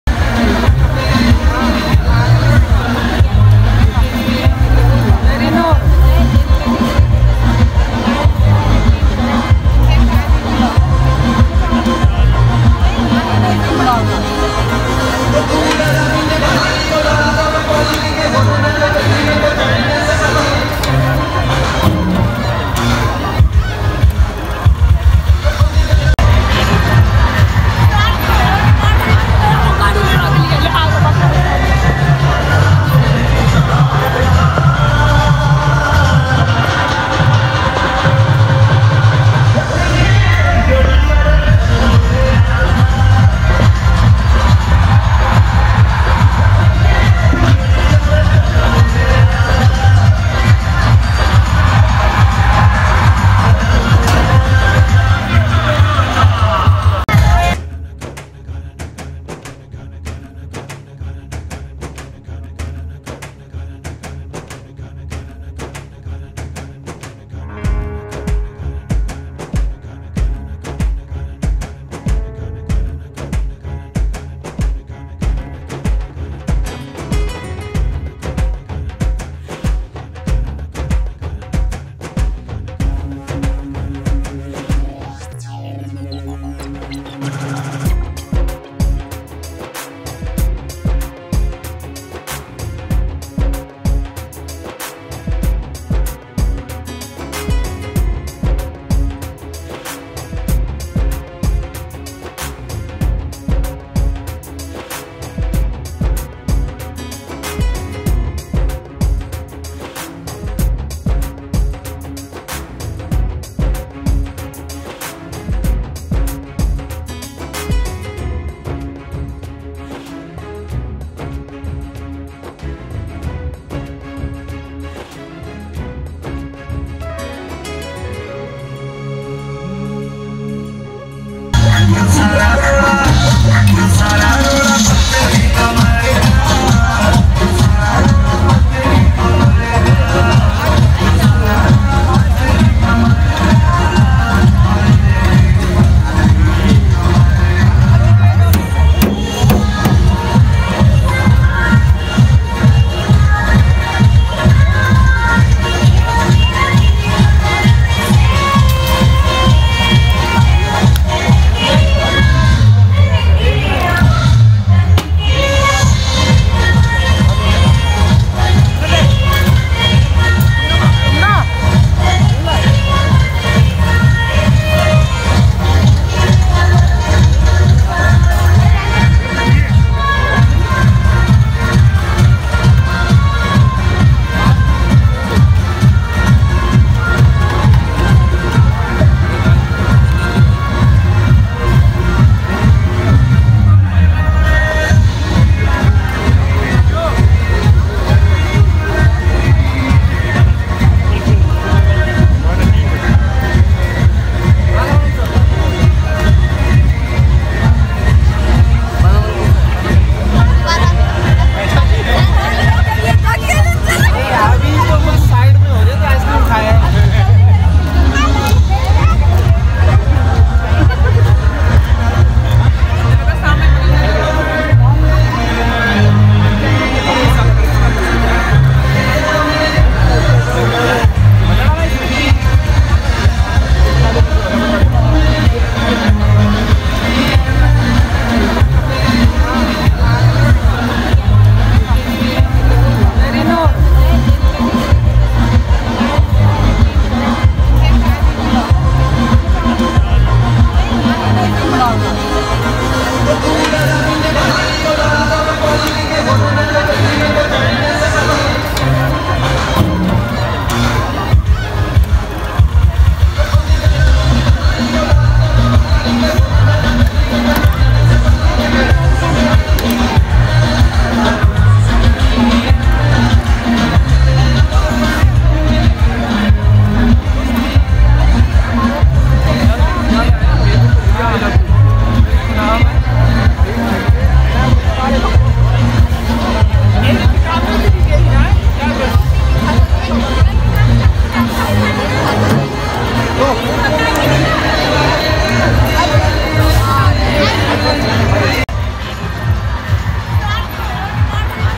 RAJASTHANI Wedding Food So Good You'll Want to Get MARRIED!
Indian Wedding Reception Food | Unlimited Buffet | Amazing rajasthani wedding food